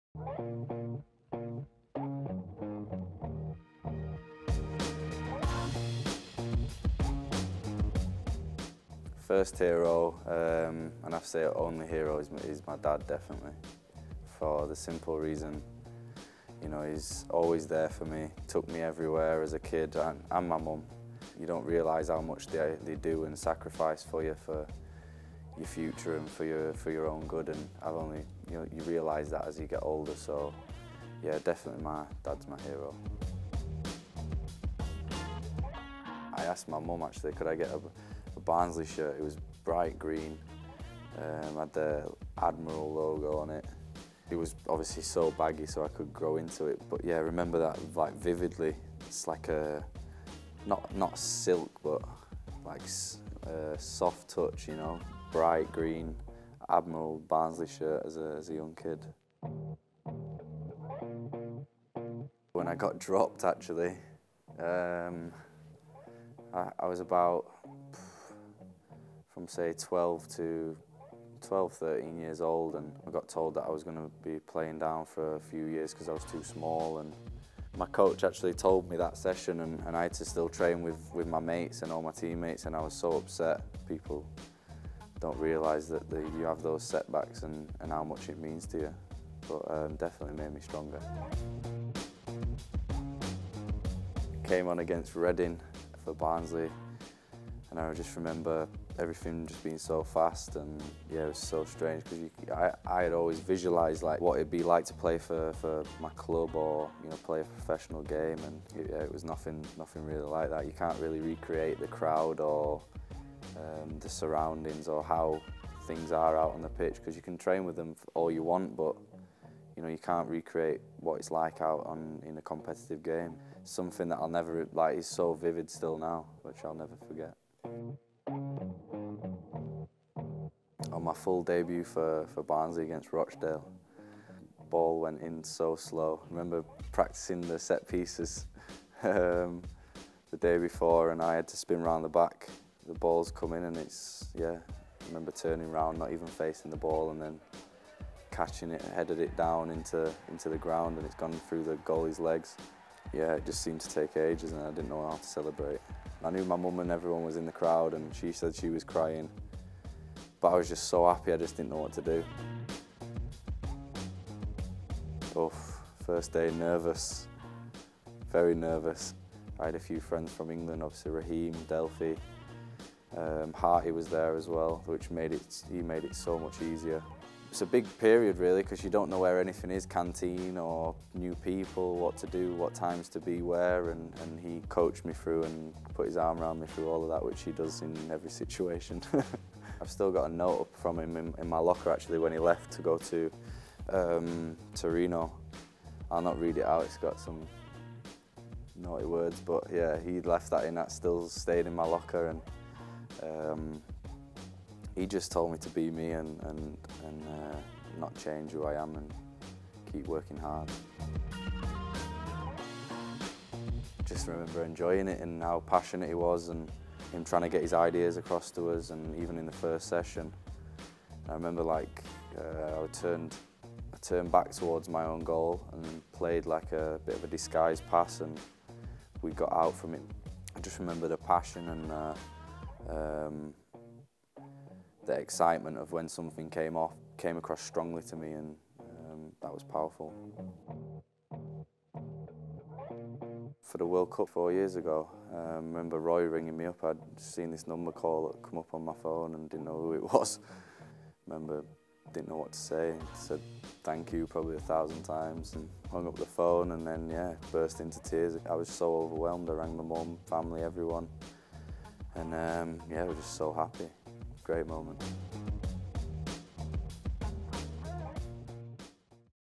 What's it like training with Pep Guardiola for the FIRST time? | John Stones
John Stones reveals the memorable FIRST moments from his career. From his debut goal, to Joe Hart's swear filled leaving note, and Pep Guardiola's first training session!